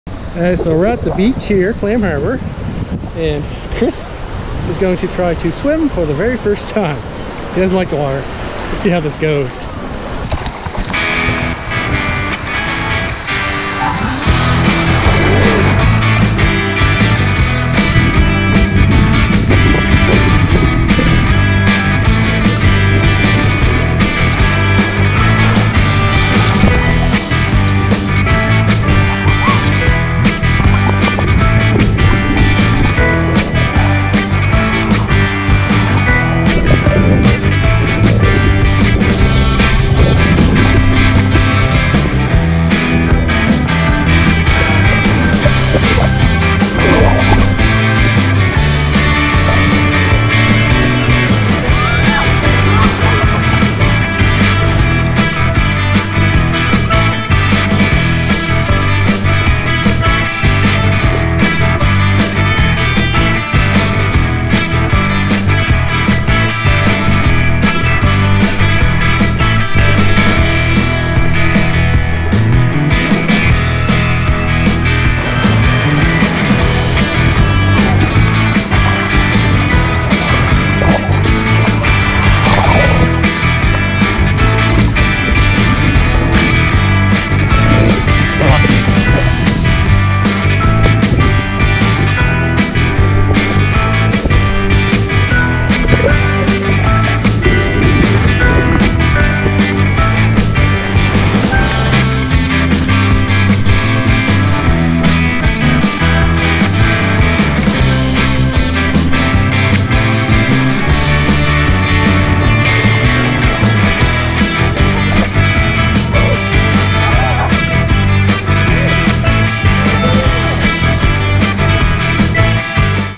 2 Adorable little dogs learning to swim
Took my  twin dogs to the ocean to teach them how to swim.  They do a great jobs